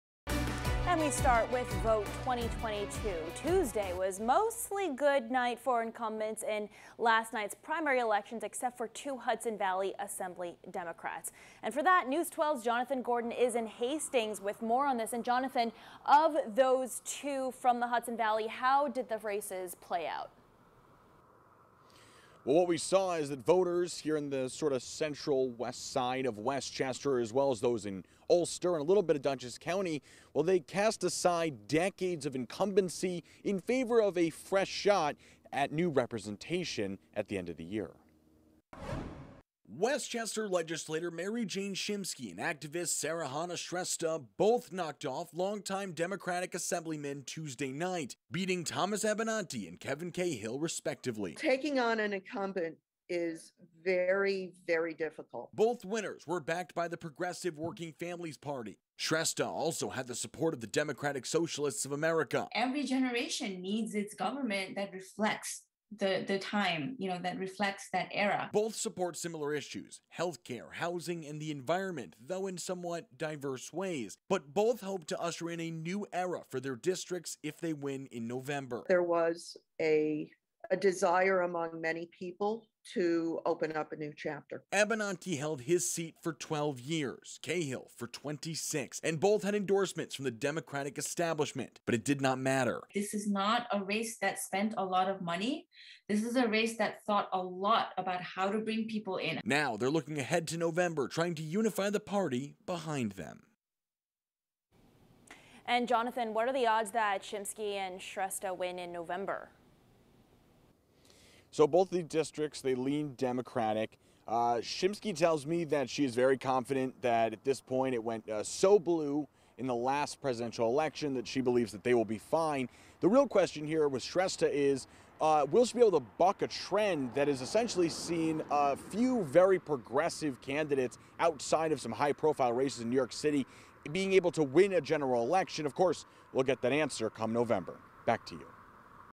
Pair of progressive Hudson Valley Democrats pull off major upsets in state Assembly primary
A pair of progressive Democrats in the Hudson Valley pulled off major upsets in Tuesday night's state Assembly primary elections.